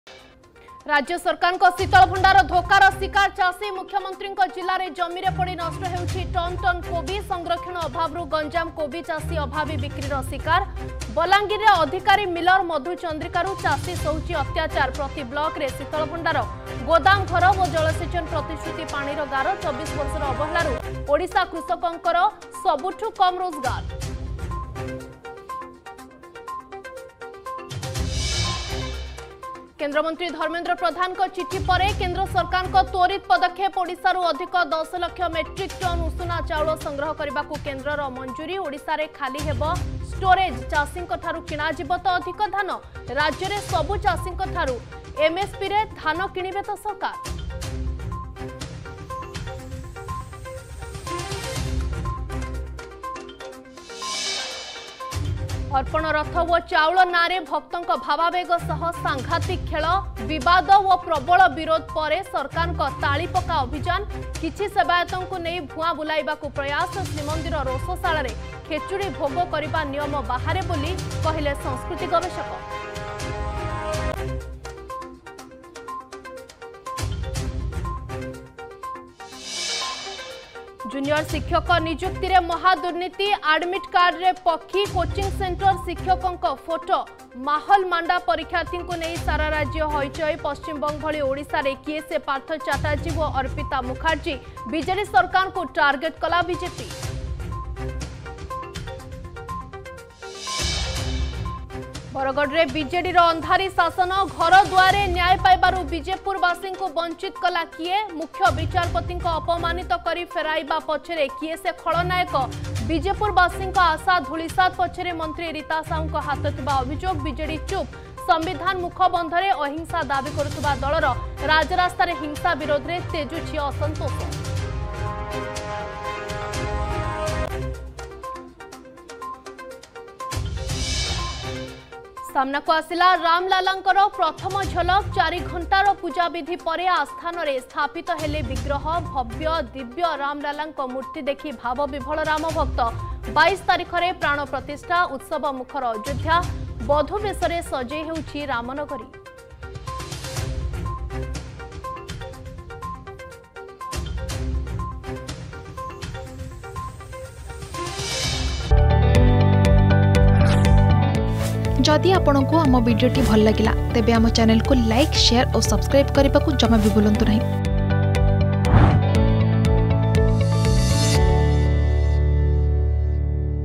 Headlines @7PM | 19 Jan 2023 | Argus News
+ରାଜ୍ୟ ସରକାରଙ୍କ ଶୀତଳଭଣ୍ଡାର ଧୋକାର ଶିକାର ଚାଷୀ
+କେନ୍ଦ୍ରମନ୍ତ୍ରୀ ଧର୍ମେନ୍ଦ୍ର ପ୍ରଧାନଙ୍କ ଚିଠି ପରେ କେନ୍ଦ୍ର ସରକାରଙ୍କ ତ୍ବରିତ ପଦକ୍ଷେପ
+ଅର୍ପଣ ରଥ ଓ ଚାଉଳ ନାଁରେ ଭକ୍ତଙ୍କ ଭାବାବେଗ ସହ ସାଂଘାତିକ ଖେଳ